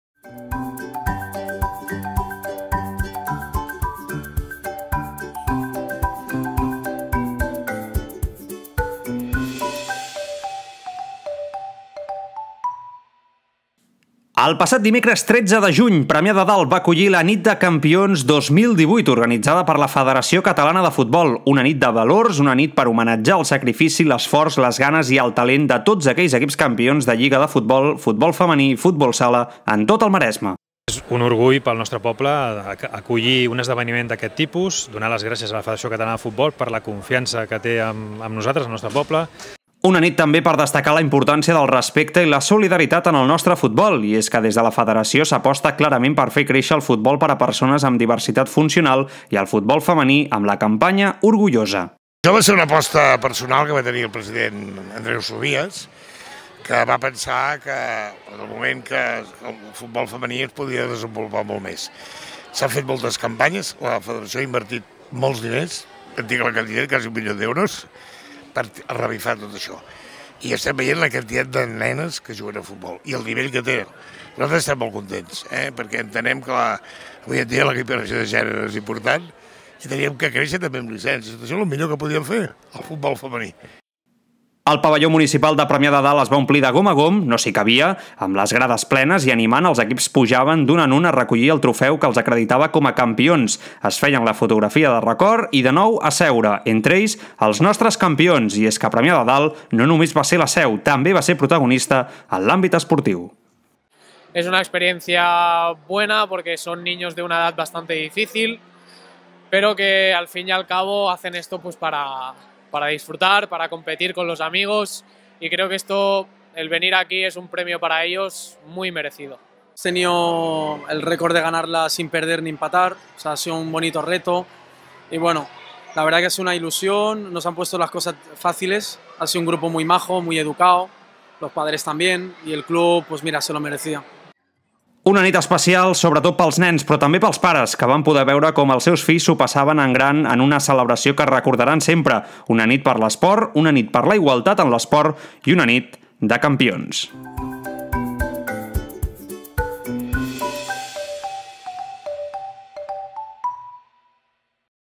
Nit de Campions 2018
Tot el treball, l'esforç, les ganes i el talent, reunit al pavelló municipal de Premià de Dalt per celebrar un any més, una nova edició de la Nit de Campions. Una celebració molt especial per tots els equips del Maresme.